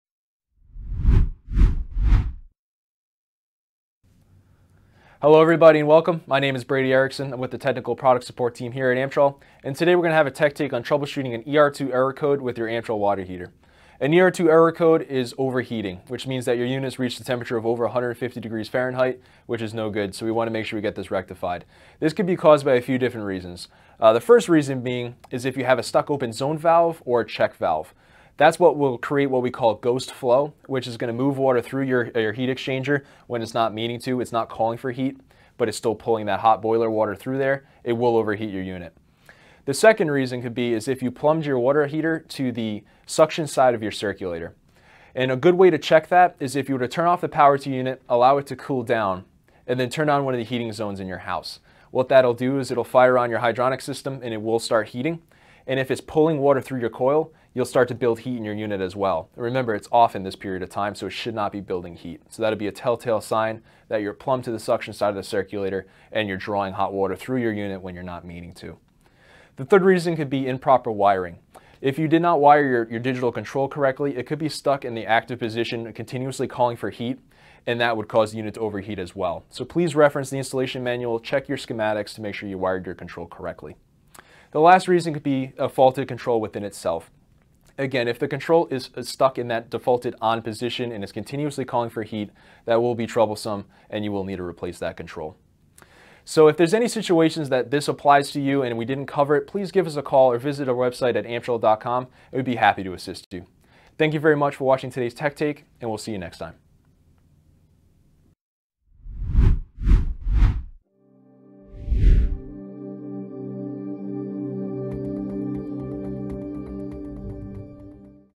Troubleshooting an "Er 2" Error - Amtrol Tech Takes
This video covers the common causes of an "Er 2" error code on your Boilermate or Hydromax digital control (Ener-g-net).

Amtrol Tech Takes are short informational videos for installers, technicians, and DIYers looking to learn more about Amtrol products and how they are used.

Chapters:
0:00 - Intro
0:14 - What is an Er 2 error
0:24 - Stuck open zone valve or check valve
0:39 - Plumbed to suction side of circulator
1:09 - Improper wiring
1:24 - Faulted control
1:36 - Closing